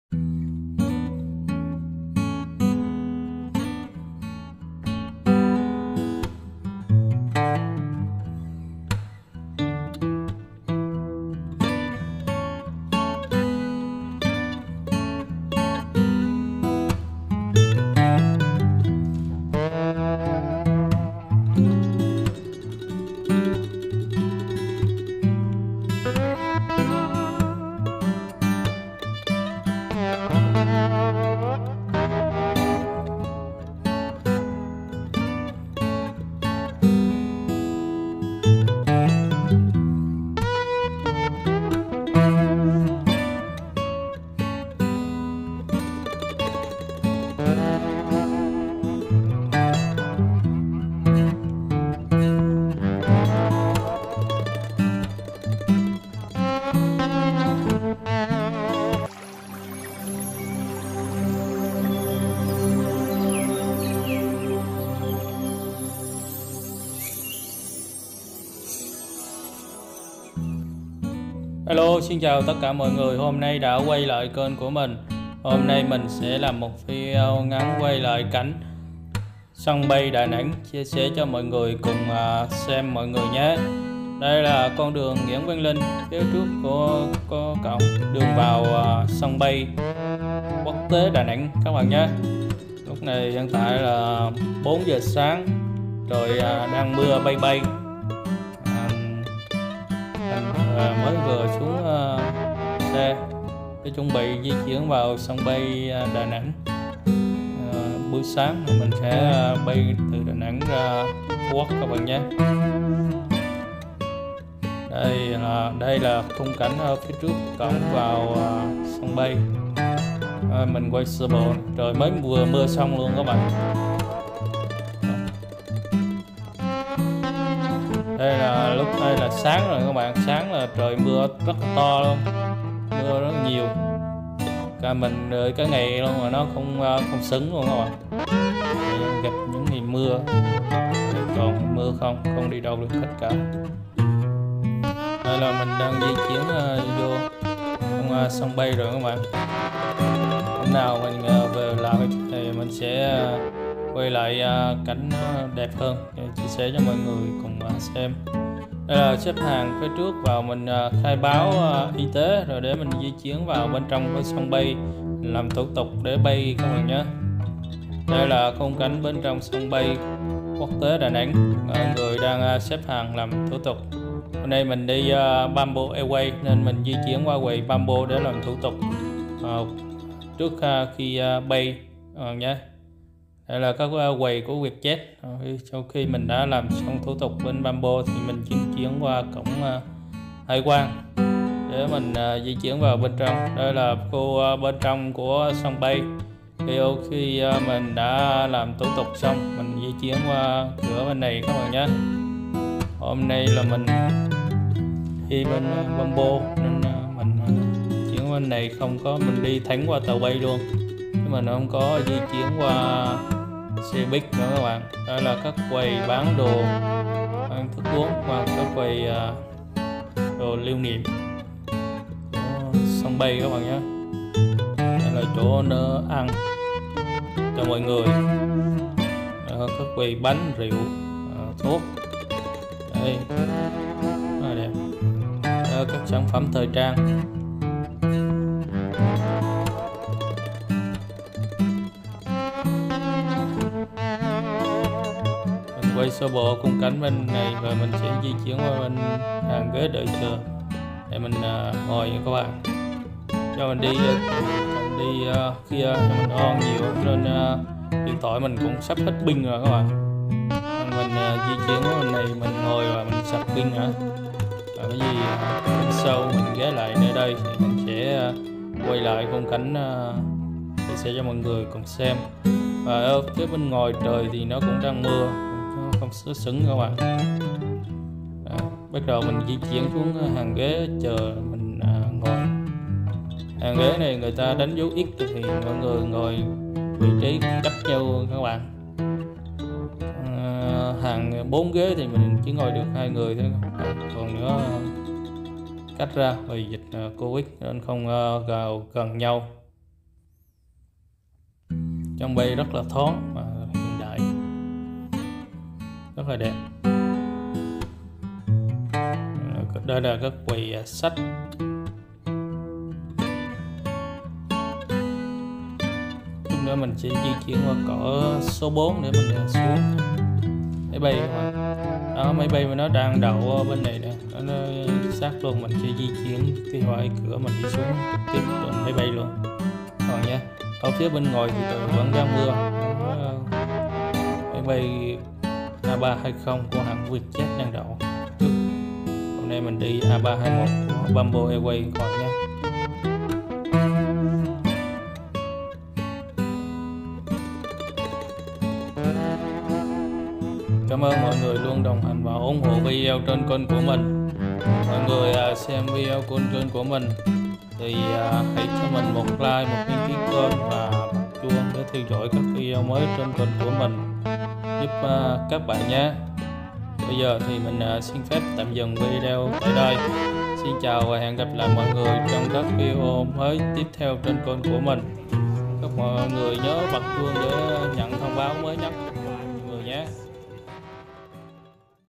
Sân Bay Quốc Tế Đà Nẵng/ Da Nang International Airport
Sân bay Quốc tế Đà Nẵng là cảng hàng không lớn nhất khu vực miền Trung - Tây Nguyên và lớn thứ ba của Việt Nam, nằm ở quận Hải Châu, cách trung tâm thành phố Đà Nẵng 3 km.